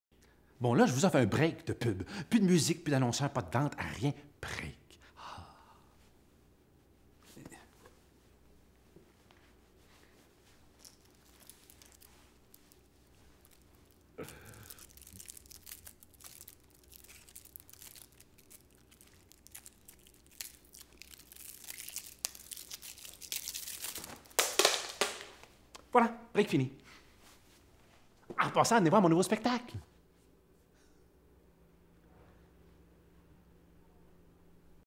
François Massicotte en spectacle à la Salle Rolland-Brunelle
François Massicotte en spectacle à la Salle Rolland-Brunelle avec son tout nouveau one-man show, Jugez-moi, le 12 décembre 2013 dès 20h. Billets en vente au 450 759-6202 ou au